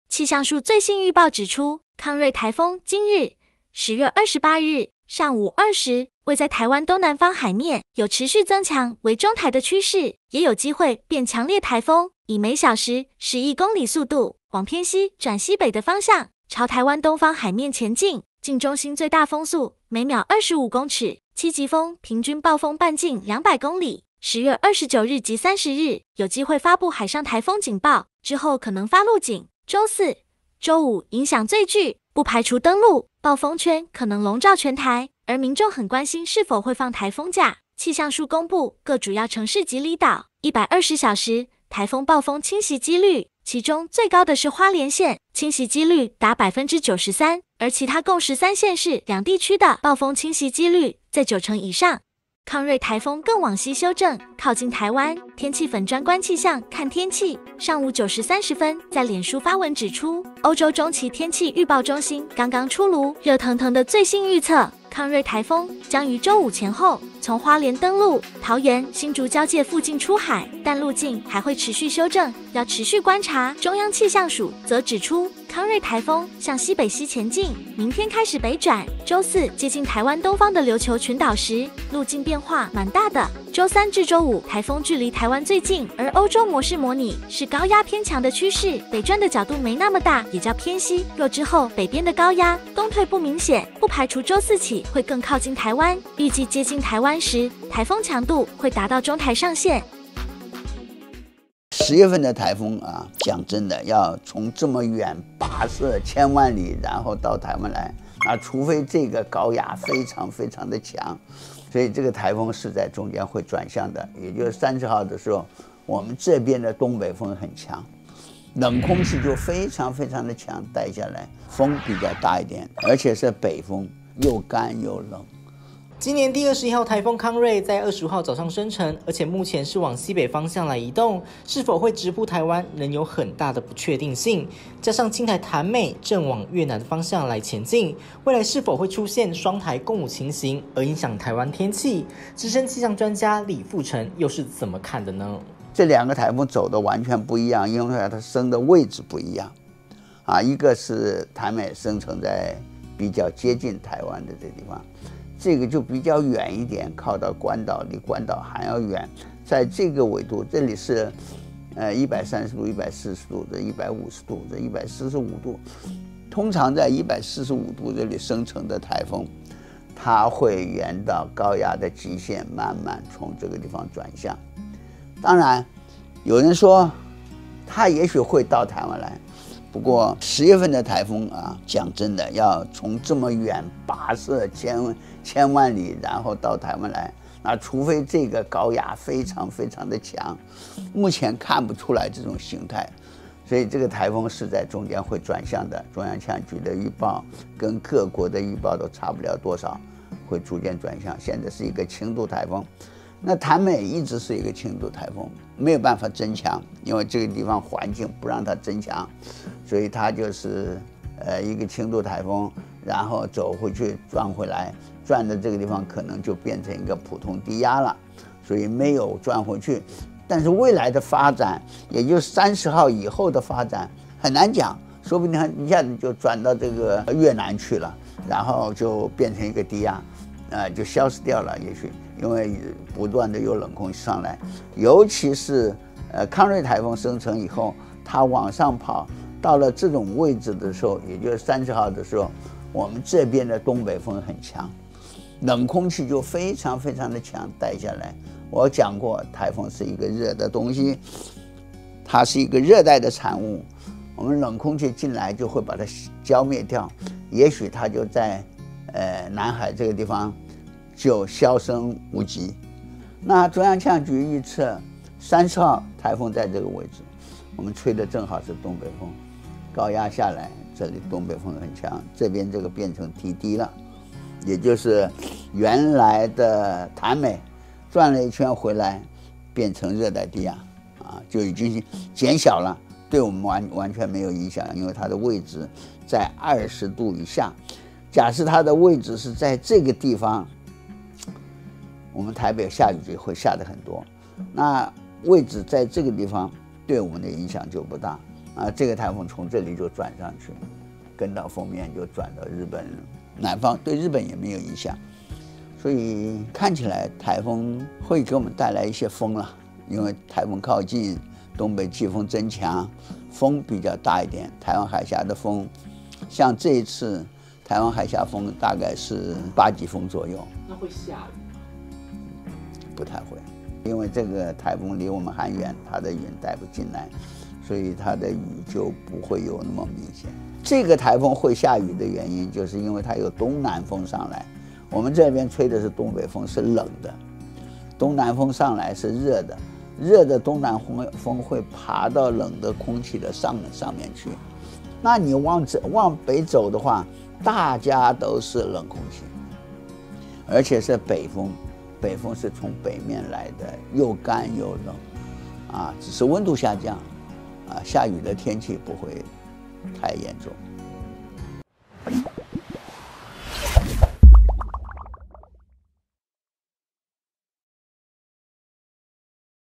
放颱風假有望？！康芮颱風路徑「直逼北台灣」 暴風圈侵襲率13縣市破90％
0:00 放颱風假有望？！康芮颱風路徑「直逼北台灣」　暴風圈侵襲率13縣市破90％
0:52 康芮颱風又更靠近台灣了！　氣象專家曝「這時」恐登陸